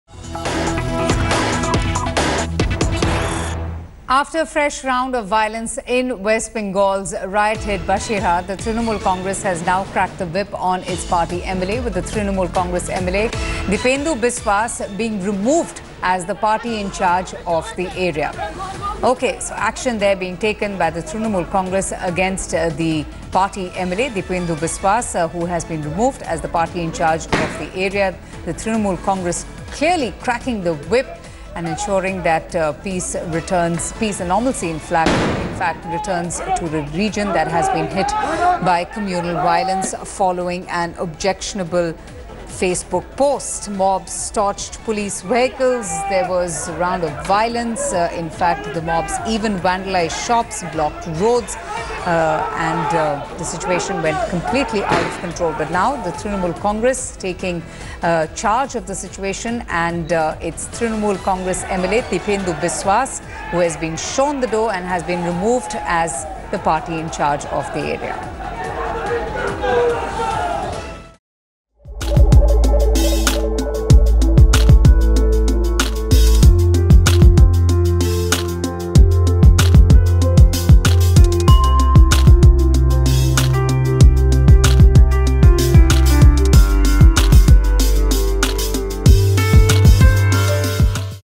TMC Removes MLA Deepednu Biswas As Area's In-charge
Following fresh violence in Basirhat TMC has removed MLA Deependu Biswas as the party's in-charge of the area